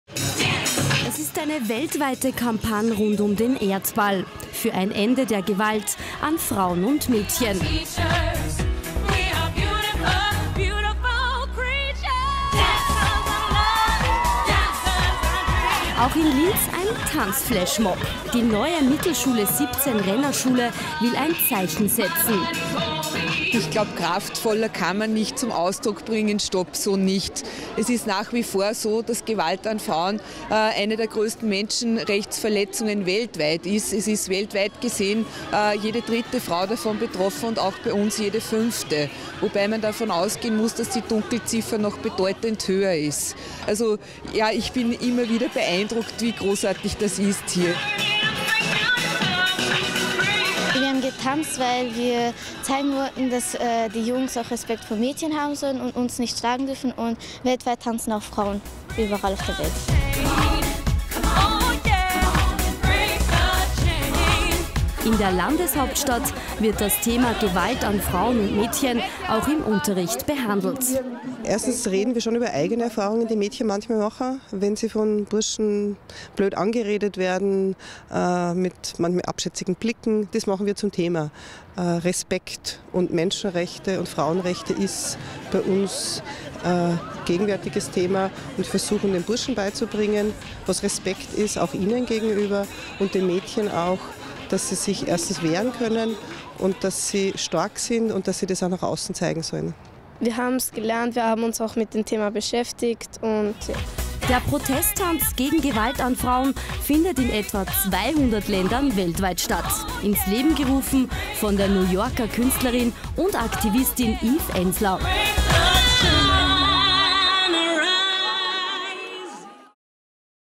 Tanz Flashmobs 200 Länder weltweit LT 1
LT1 berichtet über One Billion Rising in Linz
Schülerinnen und Schüler der NMS 17 Linz machen seit Jahren bei der weltweiten Aktion gegen Gewalt an Frauen und Mädchen am 14. Februar mit.